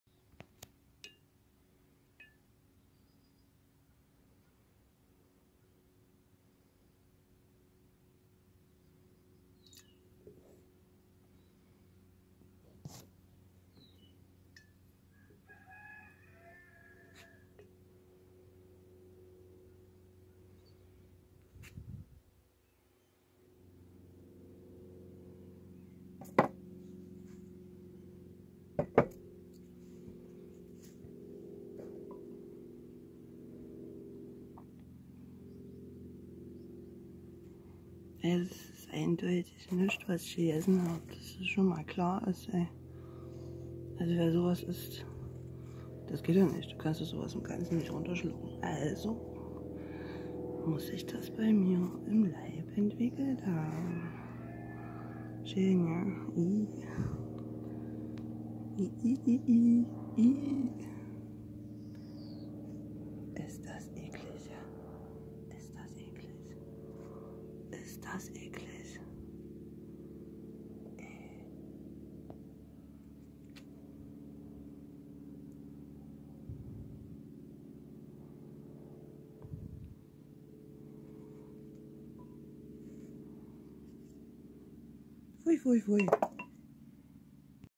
Paras??? Was hat sie da ausgka***?
Gestern morgen mal wieder was gefunden. Nach einigen Tagen Knoblauch aufs Brot...viel Knoblauch. es lohnt sich die Zeitung nich nur zu lesen...und was gibt es schöneres als sein Geschäft im Freien zu erledigen? Da fällt sowas dann auch mal auf...